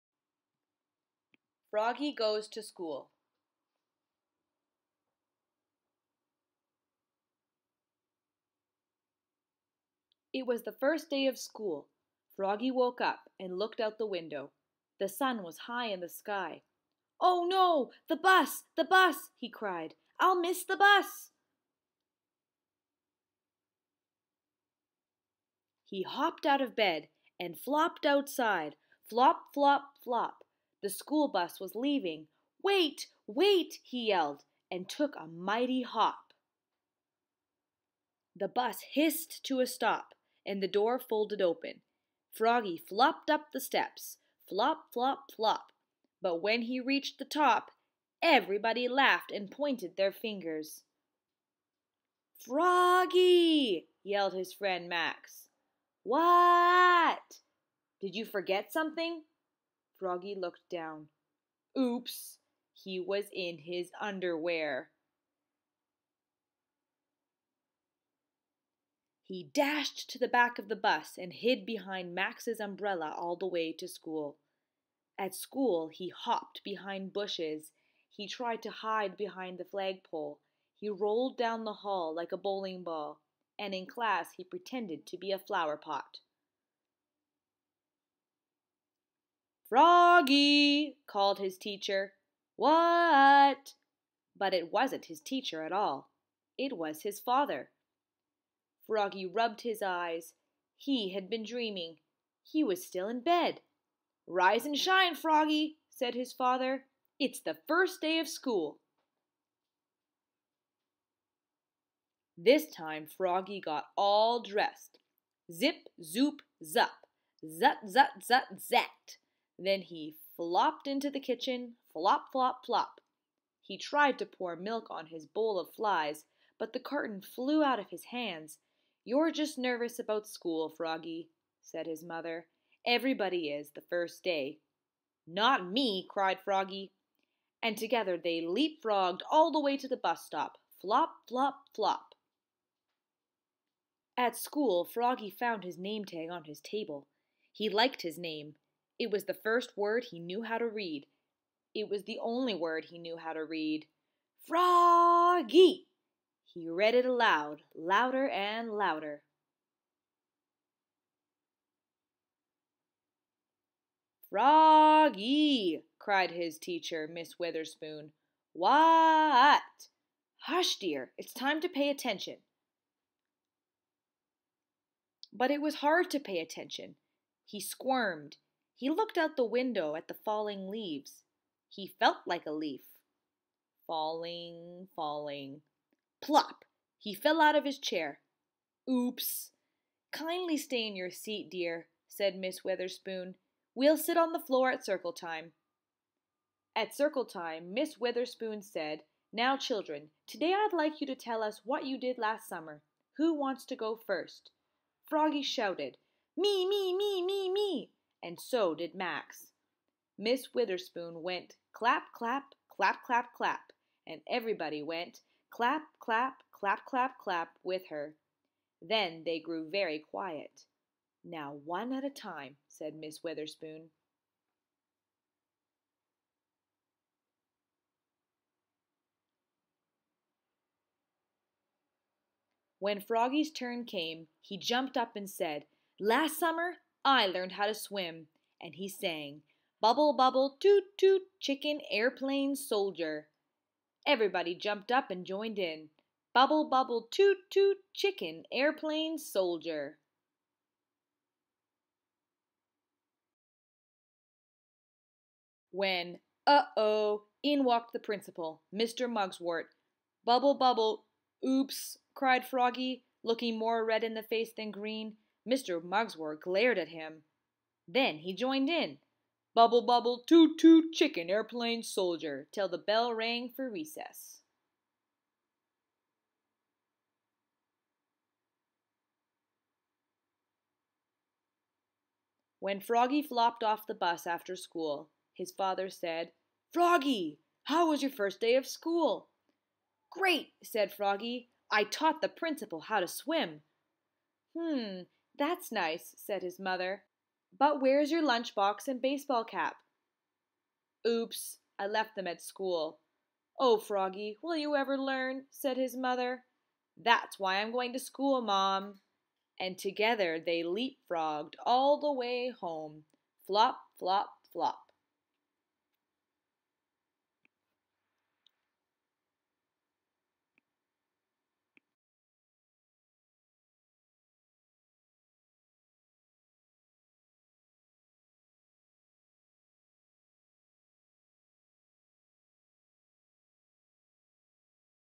Froggy Goes to School - Read Aloud by Ms. Drury
by Jonathan London (Author), Frank Remkiewicz (Illustrator)